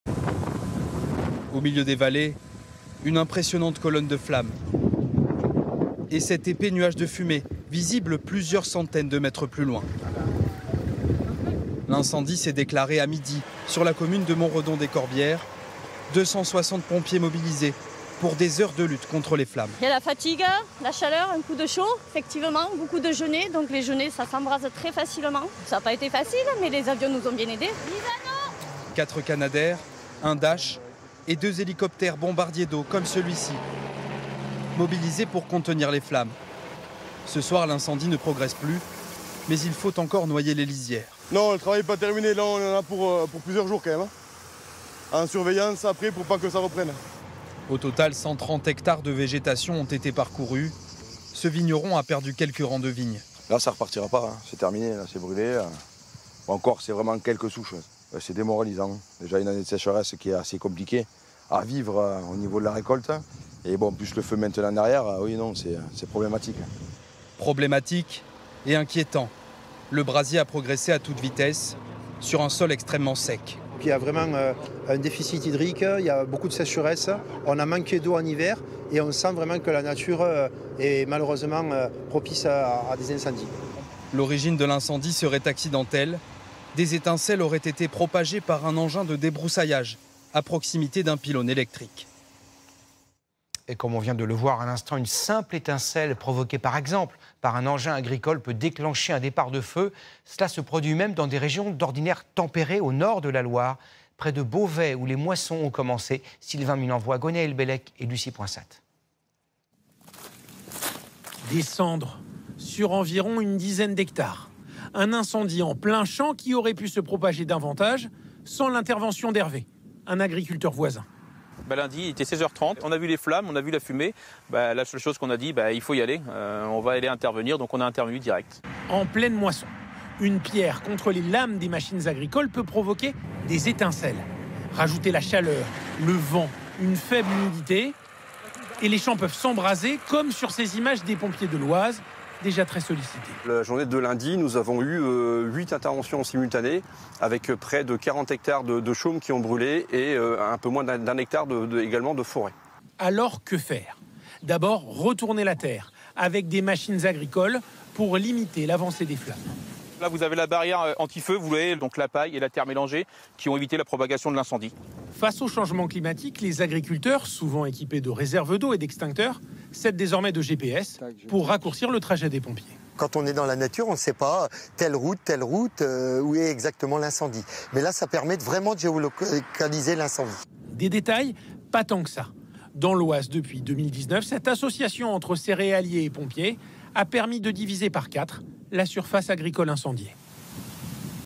Aude : des dizaines d'hectares ravagés par le feu
Un incendie s'est déclaré ce mercredi à Montredon-des-Corbières (Aude), près de Narbonne. D'après la préfecture, 100 hectares ont été parcourus par le feu.